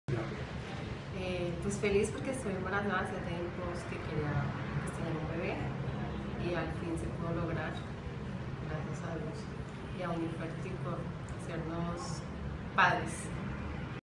TESTIMONIO DR CARLOS VIVAS Y UNFERTIL MARZO 1 2016
Nuevo testimonio de papitos felicies que hicieron realidad el sueño de ser padres. En la Unidad de Fertilidad del Tolima UNIFERTIL, con la dirección científica del Dr. Carlos Arturo Vivas en Ibagué.